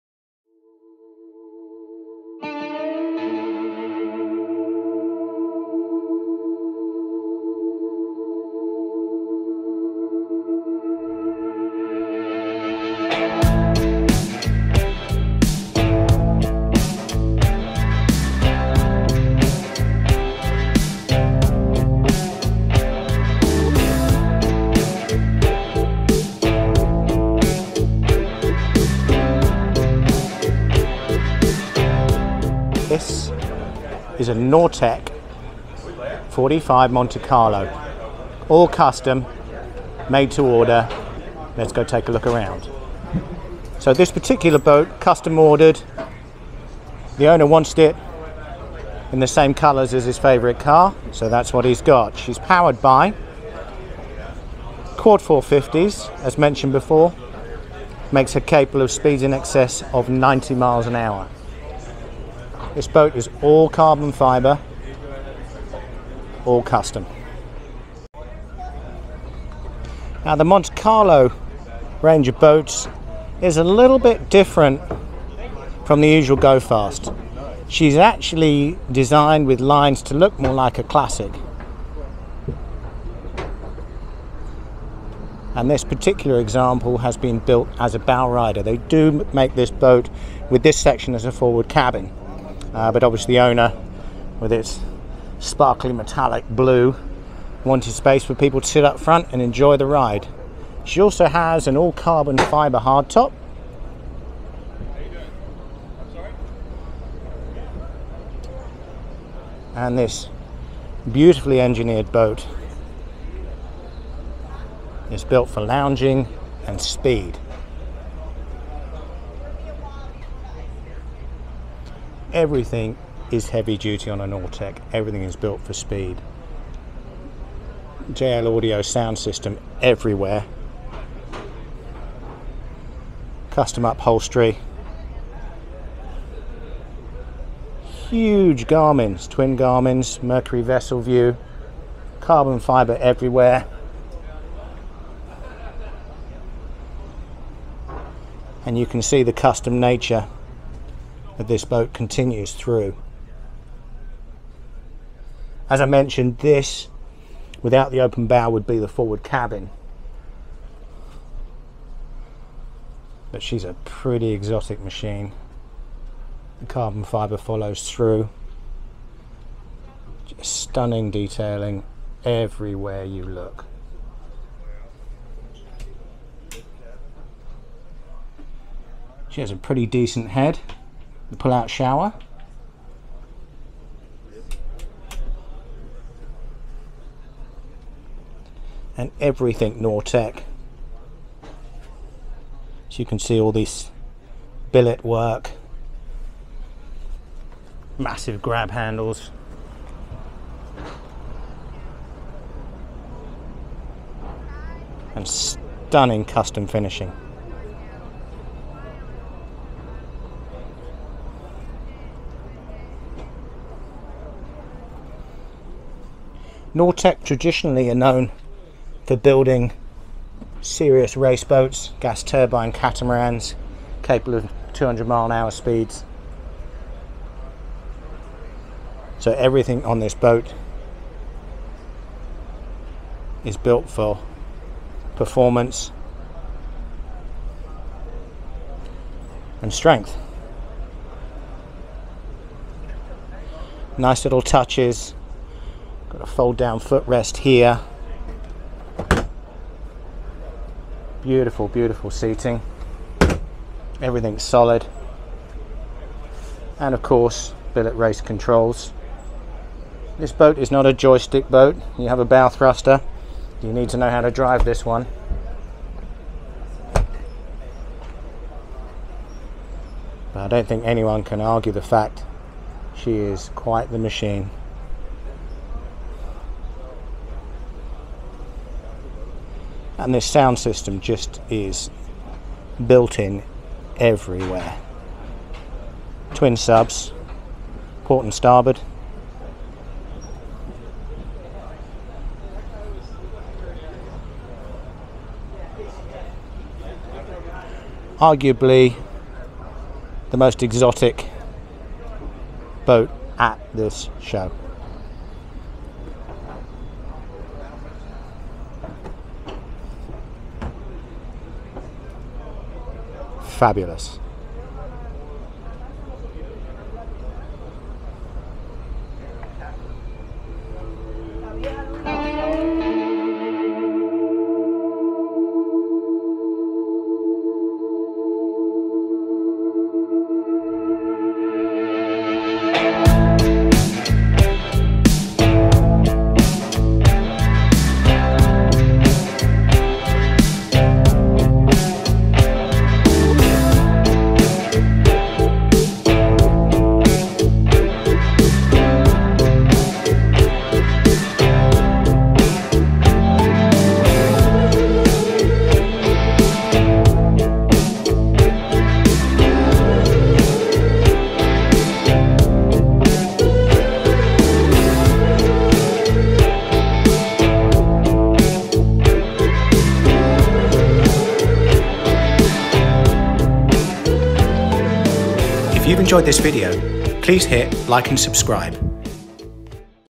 Nortech MC46 Hottest Boat at FLIBS 21????? It's incredible.... Full Walkthrough
Full Tour - Nortech MC46 Flyer $2000,000 of pure HOTNESS!